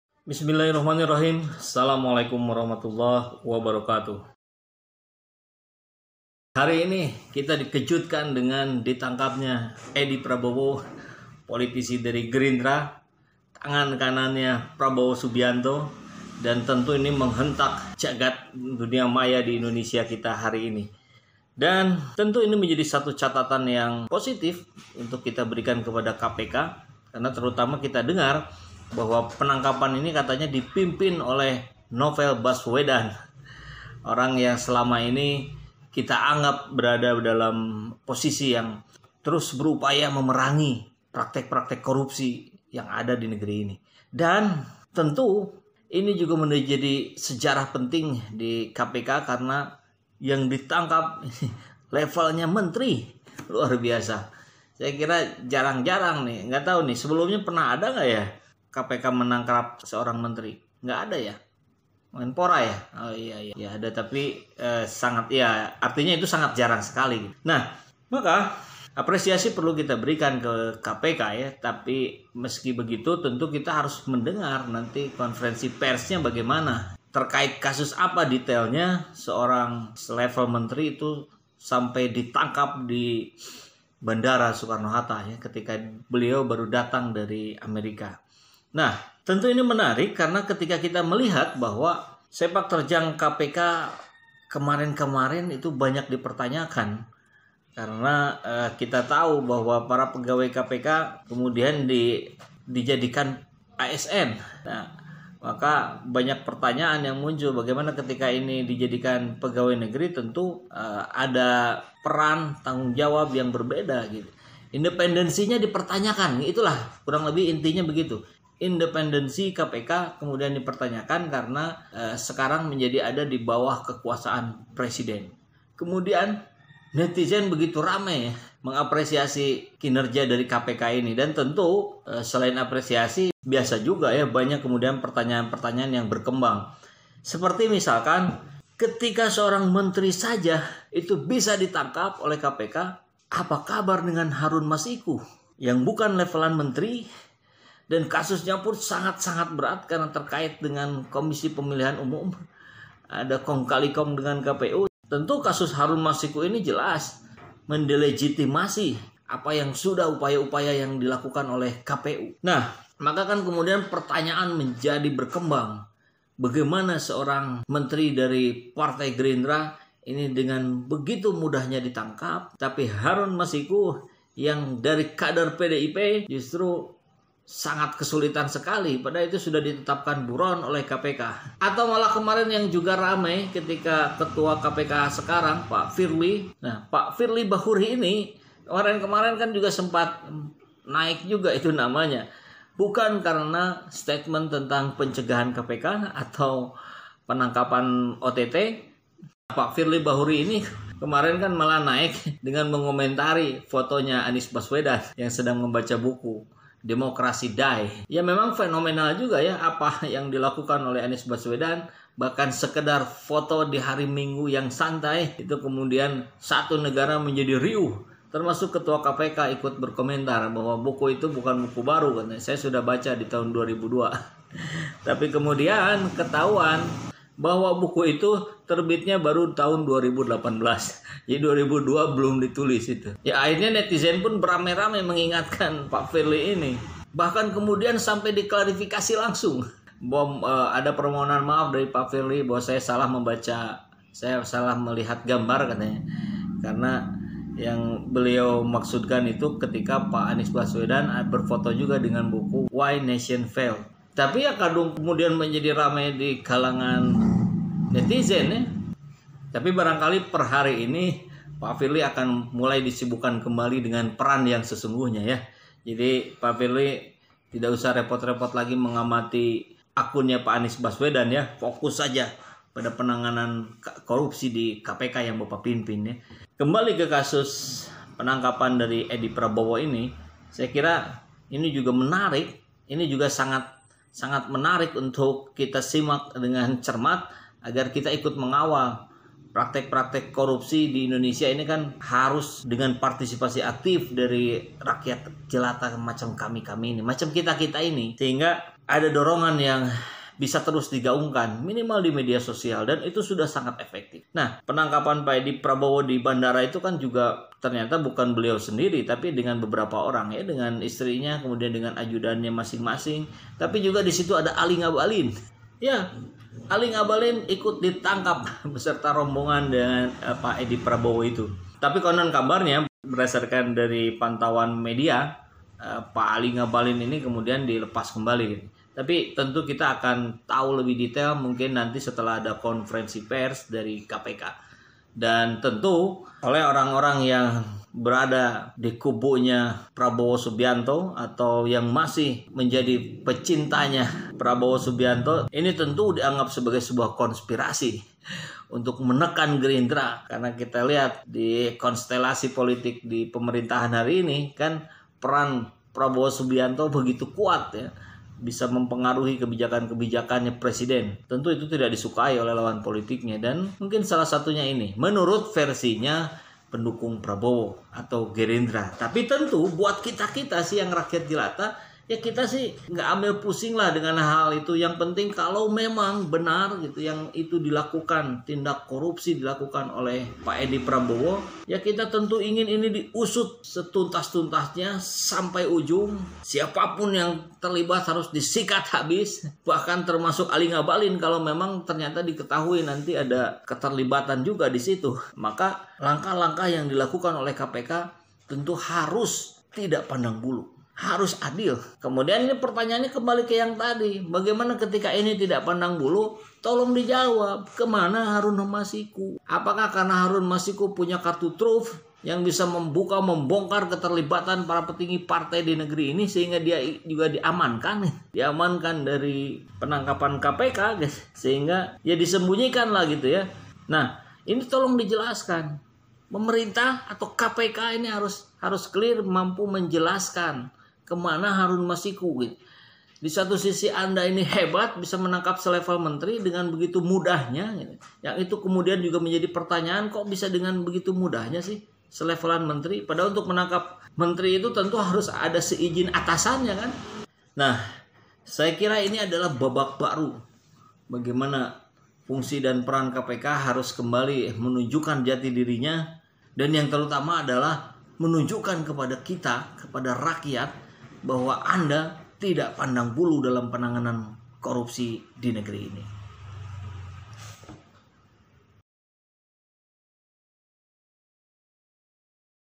KPK HEBAT !!! BERHASIL MENANGKAP SEKELAS MENTERI, MASIH BISA SENTIL ANIES. APA KABAR HARUN MASIKU ??
KPK tangkap Menteri KKP, Edhy Prabowo. Kalau setingkat menteri bisa ditangkap, berarti seharusnya yang ditangkap selanjutnya adalah Harun Masiku, yang bukan menteri bukan pejabat.

Follow Instagram, Facebook,Fan Page, & Twitter Saeful Zaman